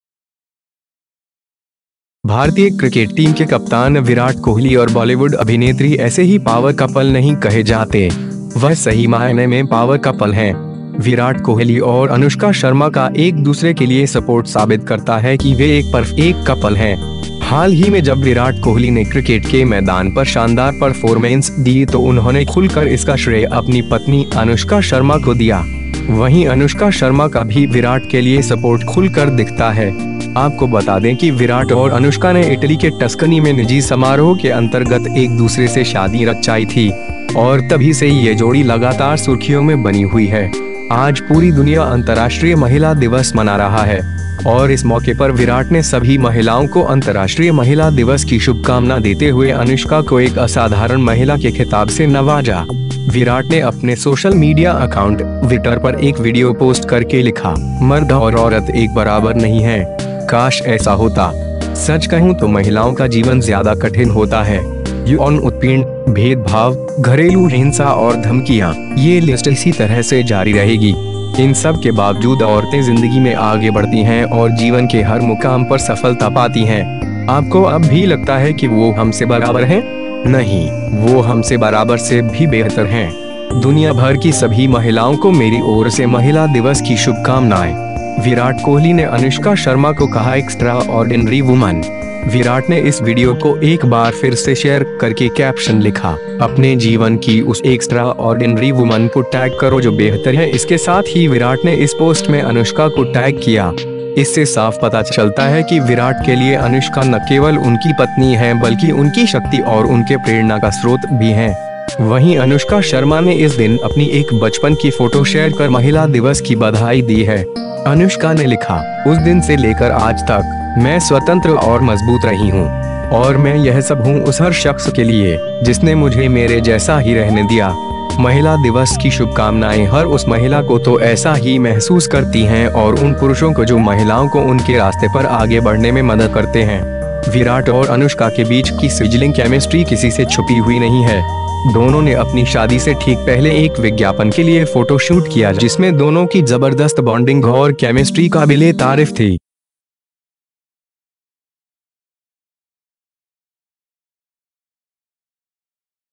Women's Day विराट कोहली के लिए अनुष्का शर्मा हैं 'एक्स्ट्रा ऑर्डिनरी वुमन'
भारतीय क्रिकेट टीम के कप्तान विराट कोहली और बॉलीवुड अभिनेत्री ऐसे ही पावर कपल नहीं कहे जाते । वह सही मायने में पावर कपल हैं । विराट कोहली और अनुष्का शर्मा का एक दूसरे के लिए सपोर्ट साबित करता है कि वे एक परफ़ेक्ट कपल हैं । हाल ही में जब विराट कोहली ने क्रिकेट के मैदान पर शानदार परफ़ोरमेंस दी तो उन्होंने खुलकर इसका श्रेय अपनी पत्नी अनुष्का शर्मा को दिया । वहीं अनुष्का शर्मा का भी विराट के लिए सपोर्ट खुलकर दिखता है । आपको बता दें कि विराट और अनुष्का ने इटली के टस्कनी में निजी समारोह के अंतर्गत एक दूसरे से शादी रचाई थी । और तभी से ये जोड़ी लगातार सुर्खियों में बनी हुई है ।

आज पूरी दुनिया अंतर्राष्ट्रीय महिला दिवस मना रहा है । और इस मौके पर विराट ने सभी महिलाओं को अंतर्राष्ट्रीय महिला दिवस की शुभकामना देते हुए अनुष्का को एक असाधारण महिला के खिताब से नवाजा ।

विराट ने अपने सोशल मीडिया अकाउंट ट्विटर पर एक वीडियो पोस्ट करके लिखा, ''मर्द और औरत एक बराबर नहीं हैं । काश ऐसा होता । सच कहूं तो महिलाओं का जीवन ज्यादा कठिन होता है । यौन उत्पीड़न, भेदभाव, घरेलू हिंसा और धमकियां...ये लिस्ट इसी तरह से जारी रहेगी । इन सबके बावजूद औरतें जिंदगी में आगे बढ़ती हैं और जीवन के हर मुकाम पर सफलता पाती हैं । आपको अब भी लगता है कि वो हमसे बराबर हैं ? नहीं, वो हमसे बराबर से भी बेहतर हैं । दुनियाभर की सभी महिलाओं को मेरी ओर से महिला दिवस की शुभकामनाएं ।"

विराट कोहली ने अनुष्का शर्मा को कहा-'एक्स्ट्रा-ऑर्डिनरी वुमन'

विराट ने इस वीडियो को एक बार फिर से शेयर करके कैप्शन लिखा, "अपने जीवन की उस एक्स्ट्रा-ऑर्डिनरी वुमन को टैग करो जो बेहतर है ।" इसके साथ ही विराट ने इस पोस्ट में अनुष्का को टैग किया । इससे साफ पता चलता है कि विराट के लिए अनुष्का न केवल उनकी पत्नी हैं बल्कि उनकी शक्ति और उनके प्रेरणा का स्रोत भी हैं ।

वहीं अनुष्‍का शर्मा ने इस दिन अपनी एक बचपन की फोटो शेयर कर महिला दिवस की बधाई दी है । अनुष्‍का ने लिखा, 'उस दिन से लेकर आज तक, मैं स्‍वतंत्र और मजबूत रही हूं.. और मैं यह सब हूं उस हर शख्‍स के लिए, जिसने मुझे मेरे जैसा ही रहने दिया... महिला दिवस की शुभकामनाएं हर उस महिला को तो ऐसा ही महसूस करती हैं और उन पुरुषों को जो महिलाओं को उनके रास्‍ते पर आगे बढ़ने में मदद करते हैं ।'

विराट और अनुष्का के बीच की सिजलिंग कैमिस्ट्री किसी से छुपी हुई नहीं है । दोनों ने अपनी शादी से ठीक पहले एक विज्ञापन के लिए फ़ोटो शूट किया जिसमें दोनों की जबरदस्त बॉंडिंग और कैमिस्ट्री काबिलेतारिफ़ थी ।